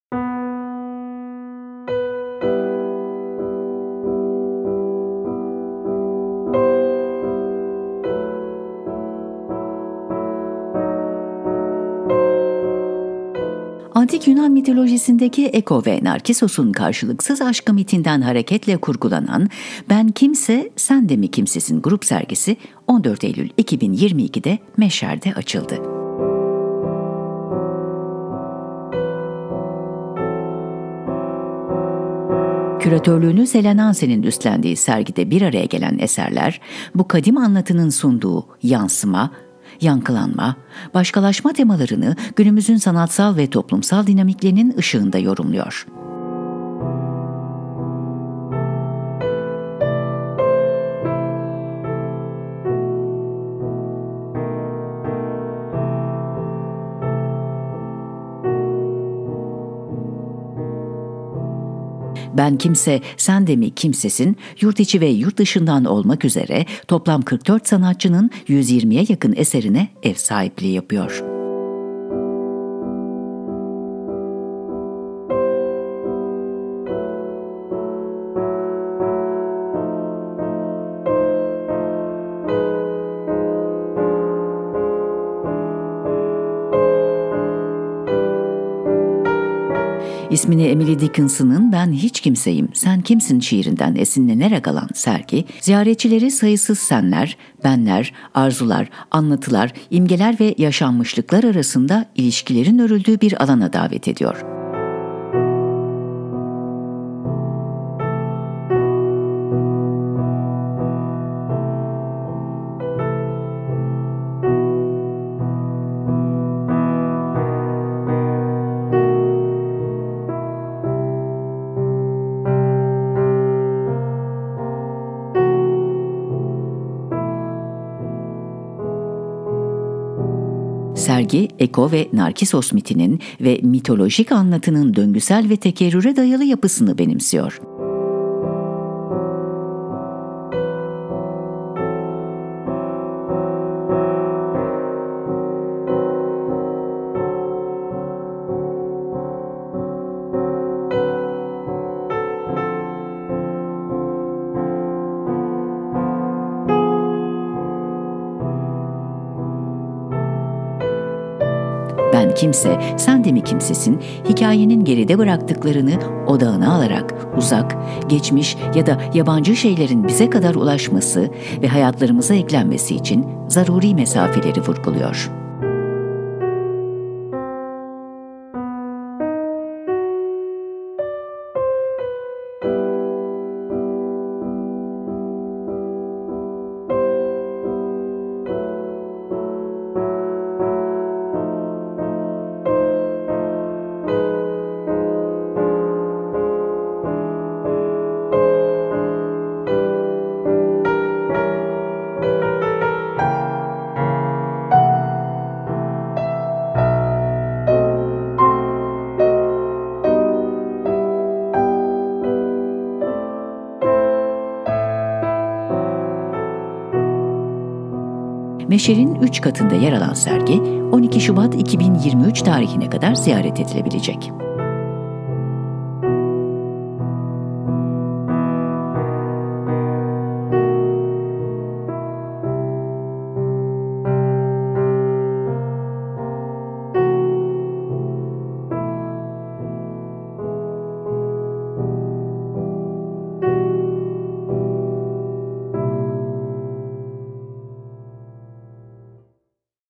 Ben Kimse. Sen de mi Kimsesin? | Meşher
Ben Kimse. Sen de mi Kimsesin?
14 Eylül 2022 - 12 Şubat 2023
Meşher

Antik Yunan mitolojisindeki Ekho ve Narkissos’un karşılıksız aşkı mitinden hareketle kurgulanan Ben Kimse. Sen de mi Kimsesin? grup sergisi 14 Eylül 2022’de Meşher’de açıldı.Küratörlüğünü Selen Ansen’in üstlendiği sergide bir araya gelen eserler, bu kadim anlatının sunduğu yansıma, yankılanma, başkalaşma temalarını günümüzün sanatsal ve toplumsal dinamiklerinin ışığında yorumluyor. Ben Kimse. Sen de mi Kimsesin? yurt içi ve yurt dışından olmak üzere toplam 44 sanatçının 120’ye yakın eserine ev sahipliği yapıyor; ödünç alınan eserlerin yanı sıra, Vehbi Koç Vakfı desteğiyle bu sergi bağlamında çeşitli mecralarda üretilen yapıtlar da yer alıyor. Meşher’in üç katında yer alan sergi, 12 Şubat 2023 tarihine kadar ziyaret edilebilecek.

İsmini Emily Dickinson’ın “Ben Hiç Kimseyim! Sen Kimsin?” şiirinden esinlenerek alan sergi, ziyaretçileri sayısız sen(ler), ben(ler), arzular, anlatılar, imgeler ve yaşanmışlıklar arasında ilişkilerin örüldüğü bir alana davet ediyor. Sergi, Ekho ve Narkissos mitinin ve mitolojik anlatının döngüsel ve tekerrüre dayalı yapısını benimsiyor. Ben Kimse. Sen de mi Kimsesin? hikâyenin geride bıraktıklarını odağına alarak uzak, geçmiş ya da yabancı şeylerin bize kadar ulaşması ve hayatlarımıza eklenmesi için zaruri mesafeleri vurguluyor.

Sanatçılar: Marina Abramović, Erol Akyavaş, Dieter Appelt, Koray Ariş, Eylül Aslan, Levent Aygül, Mehtap Baydu, Deniz Bilgin, Handan Börüteçene, Elina Brotherus, Betty Bui, Claude Cahun,             Aslı Çavuşoğlu, Laurence Demaison, Lee Friedlander, Ryan Gander, Nan Goldin,   Jessica Harrison,         Camille Henrot, Charles Holland & Elly Ward, Talbot Hughes, John Isaacs, Fatoş İrwen, Gizem Karakaş, Nermin Kura, Marcantonio Raimondi Malerba, Bevis Martin & Charlie Youle, Radenko Milak, İz Öztat, İz & Ra, Evan Penny, Gerhard Richter, Necla Rüzgar, Stéphanie Saadé,    Fabrice Samyn, Jenny Saville & Glen Luchford, Yusuf Sevinçli, Yaşam Şaşmazer, Ayça Telgeren,  Defne Tesal, VOID